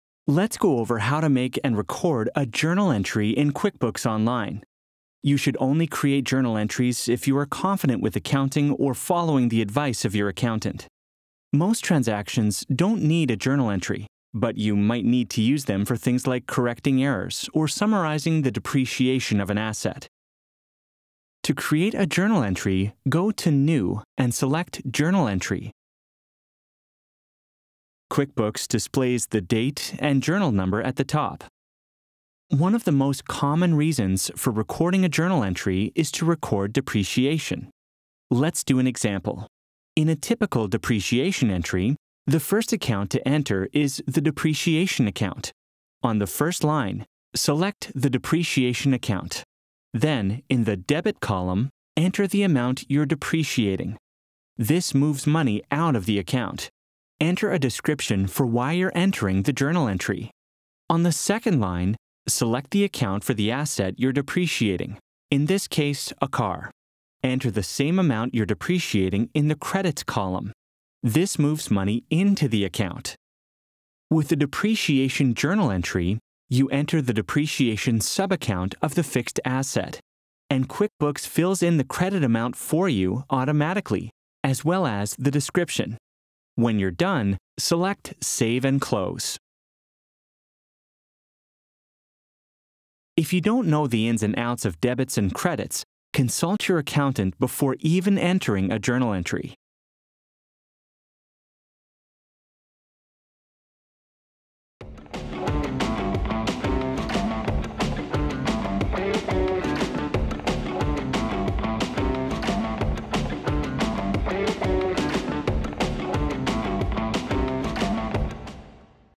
How to Make and Record a Journal Entry in QuickBooks Online
Let’s go over how to make and record a journal entry in QuickBooks Online. You should only create journal entries if you are confident with accounting or following the advice of your accountant.

Most transactions don’t need a journal entry, but you might need to use them for things like correcting errors or summarizing the depreciation of an asset.